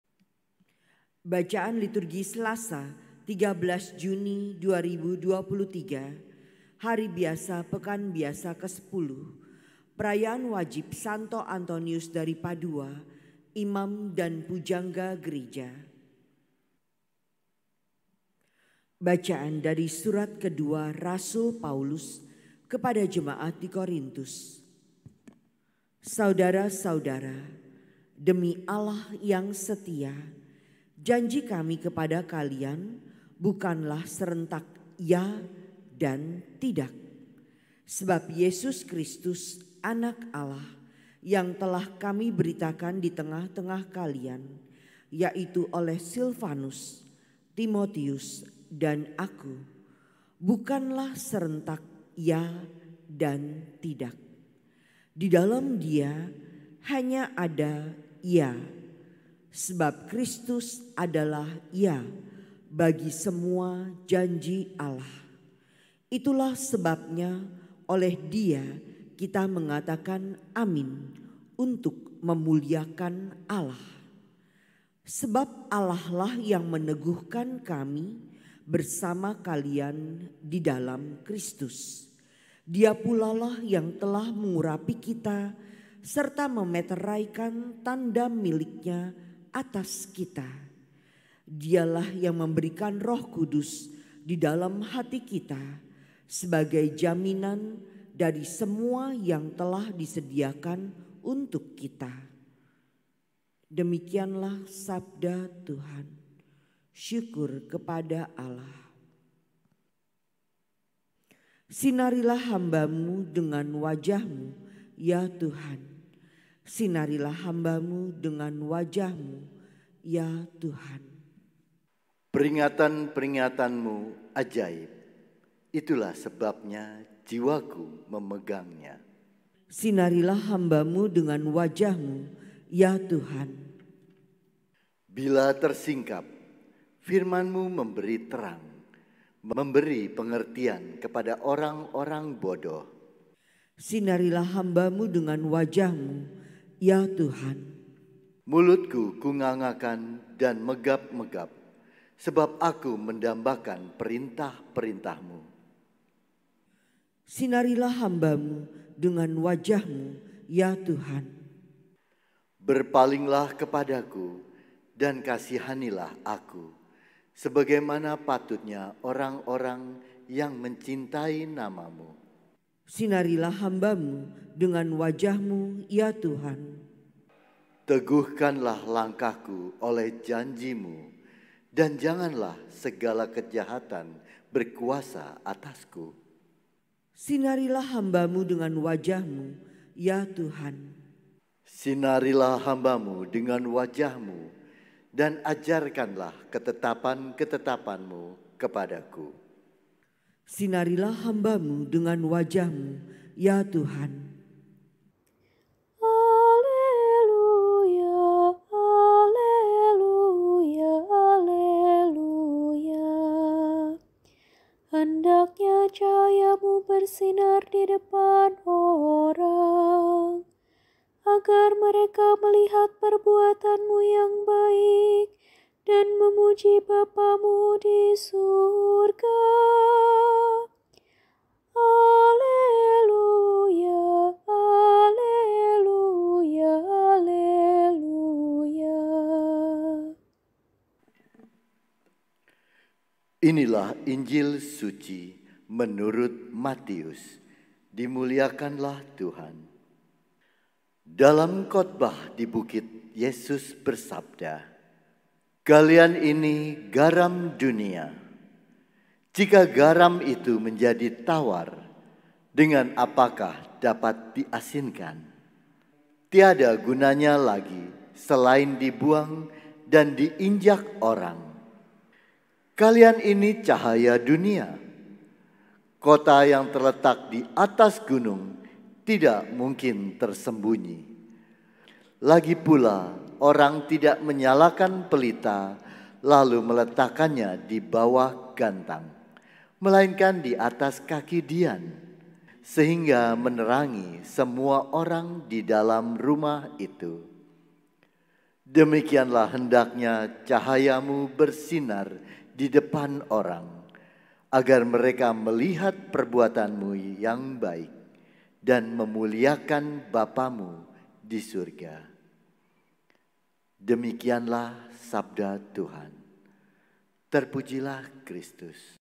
Bacaan Liturgi Selasa 13 Juni 2023 - Hari Biasa Pekan Biasa X/ CATUR
Kalender Liturgi 13 Juni 2023
Selasa Pekan Biasa X
PW S. Antonius dari Padua, Imam dan Pujangga Gereja
Bacaan I: 2Kor 1: 18-22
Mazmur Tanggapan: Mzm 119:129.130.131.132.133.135
Bait Pengantar Injil: Mat 5:16
Bacaan Injil: Mat 5:13-16